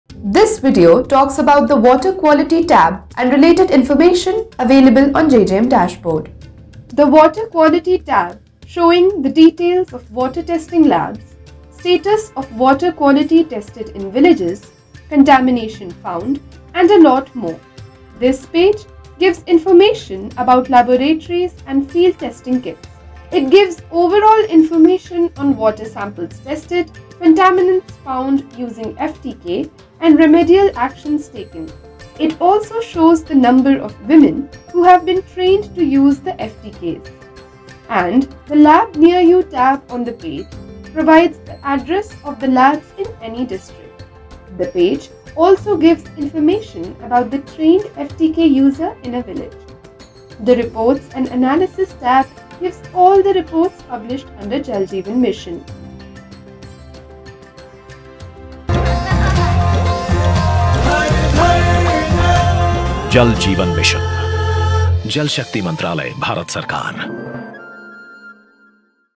JalJeevanMission tutorial: Water Quality_#HarGharJal
In the series of JJM Totorial videos, this 5th video will help you understand how to access the Water Quality tab in JJM dashboard. In addition, you can also see the categories, progress, and other district details.